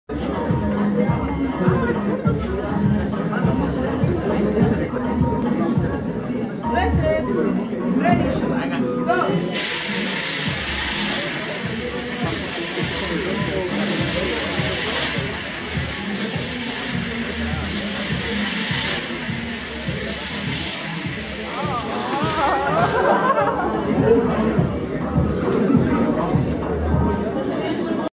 Seguidor de linea velocista falla, Reto Veracruz por la Ciencia
Cuando tu robot corre bien pero se le aloca a la mera hora...